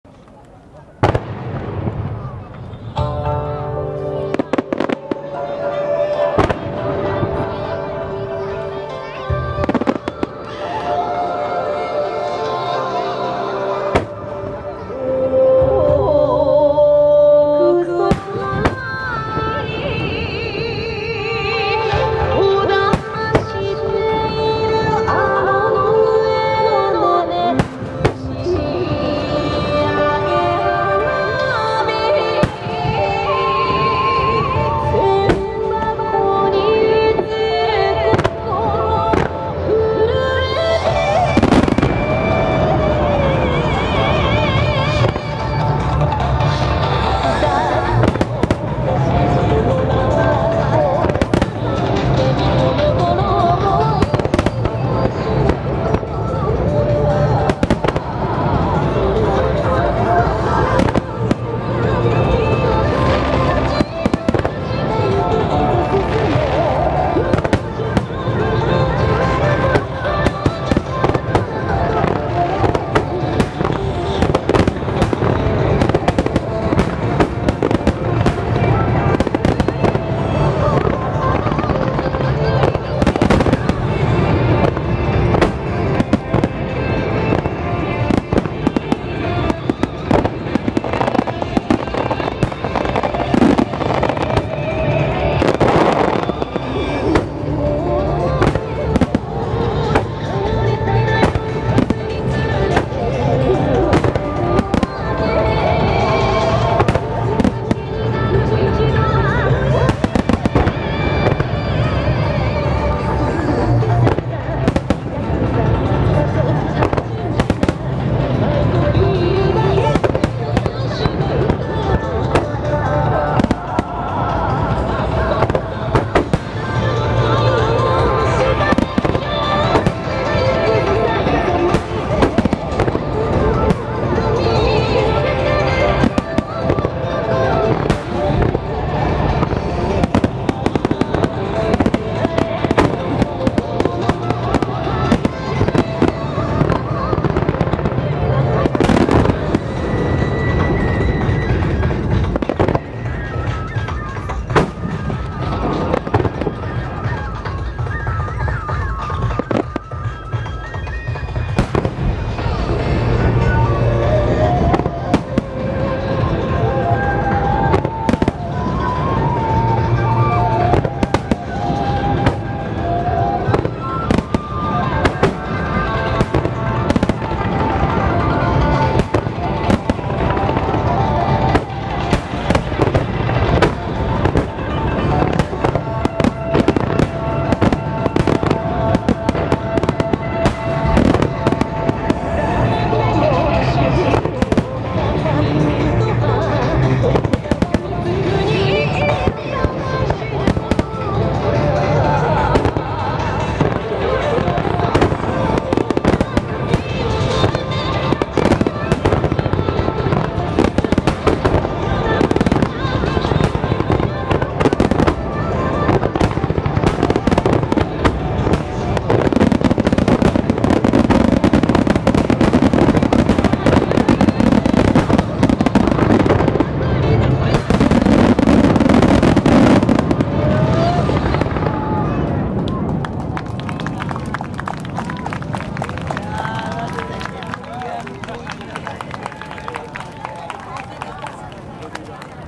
【ケムケム残念】水戸偕楽園花火大会2024 フィナーレ 超特大ミュージックワイドスターマイン【HDR】
担当　野村花火工業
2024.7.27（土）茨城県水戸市